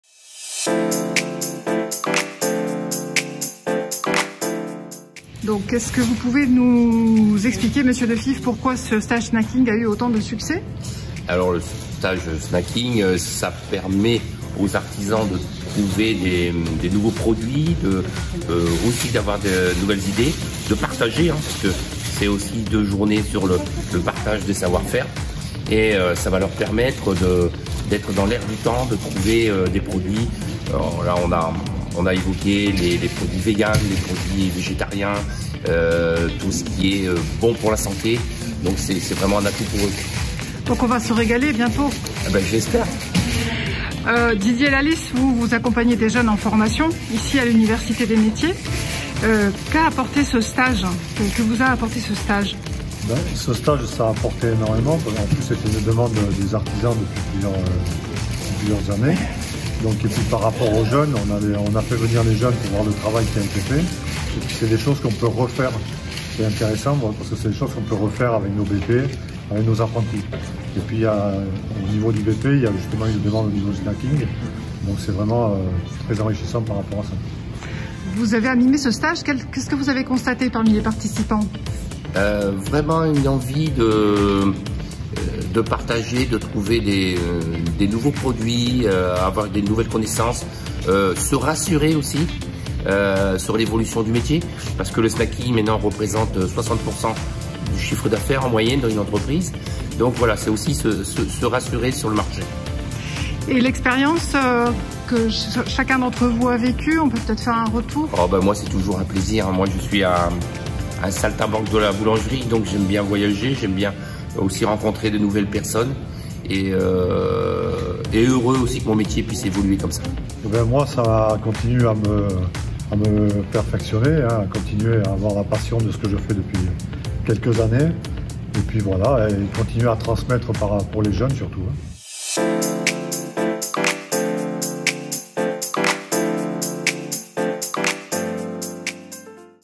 Stage snac pour les pros
Les artisans métiers de bouche peuvent se former en local grâce à la Cité du goût et des saveurs des Pyrénées-Atlantiques.

Ils s'améliorent sur de nouvelles techniques, repartent avec des idées de recettes, et échangent avec des formateurs d'expérience.
Interview de Joël Defives et Didier Lalys lors du stage sur la pratique professionnelle du snacking à l'Université des Métiers de Pau.